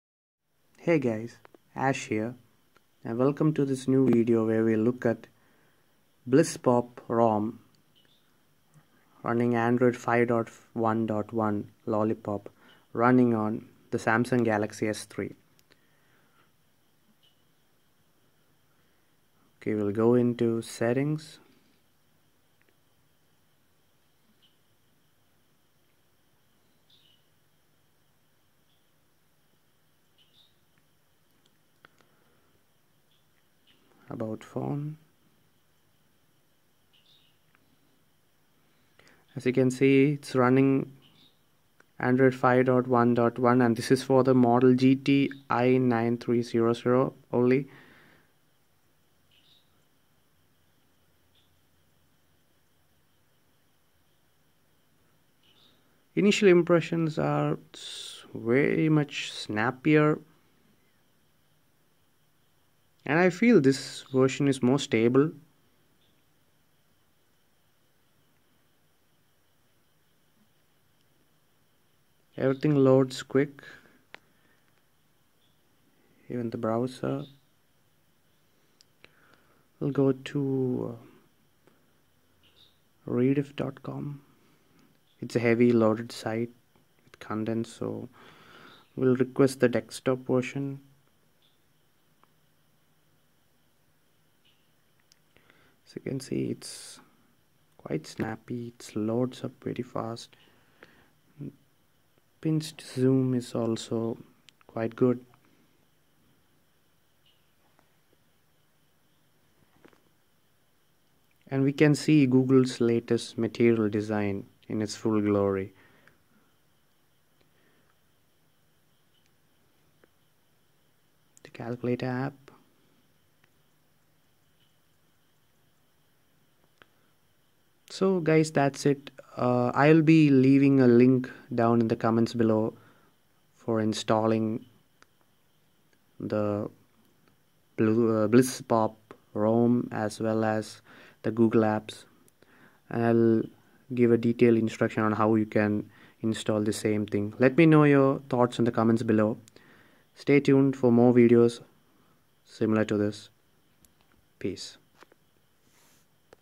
How to Install Android 5.1.1 Lollipop on Galaxy S3 I9300 Bliss Pop 3.2
Bliss Pop 3.2 ~ Android Lollipop 5.1.1 running on Samsung Galaxy S3 : GT-I9300 model.